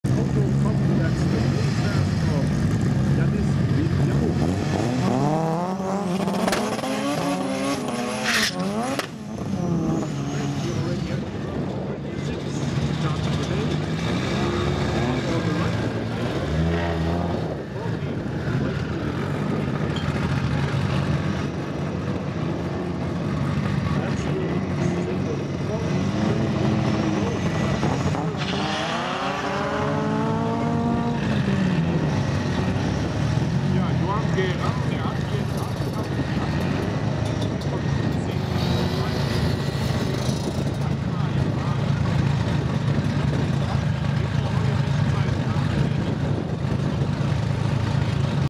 Sneaky Tuning "Boost-LEE" (Winesh Akloe) - 12.29 sec @ 1/4mile - Das Drag Day #12 - Bitburg
Sneaky Tuning "Boost-LEE" (Winesh Akloe) runs 12.29 sec @ 1/4 mile - Das Drag Day #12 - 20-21 sep 2014 Bitburg (GER)